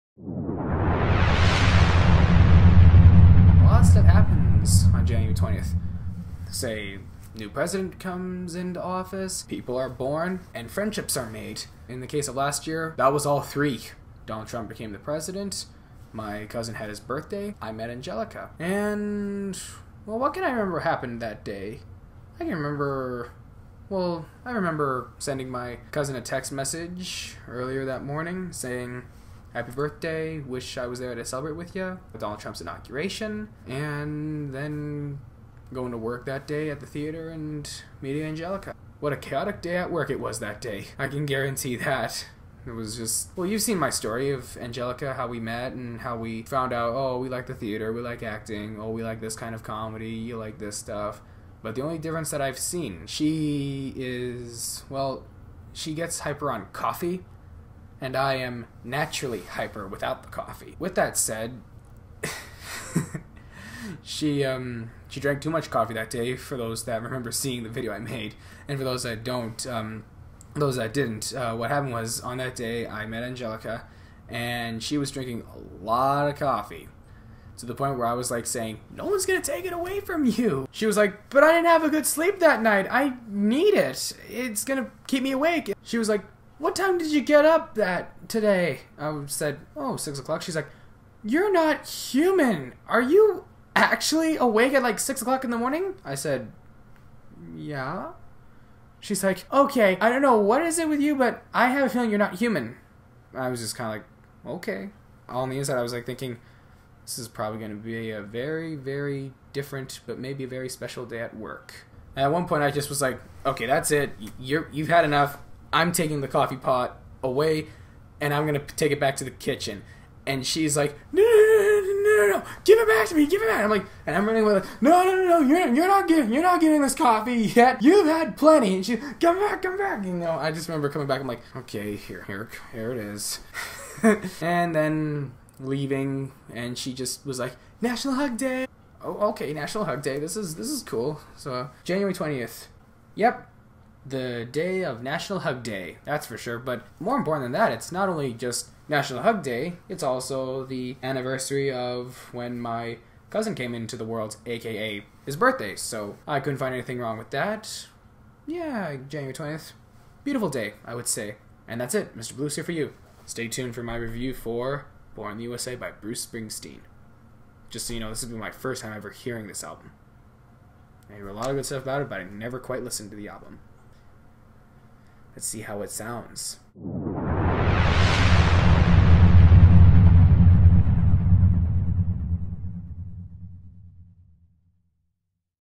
January 20th 2017
Angelica was a good friend. It has been a full year since we met. I recorded this tape on January 20th, 2018 and the anniversary since meeting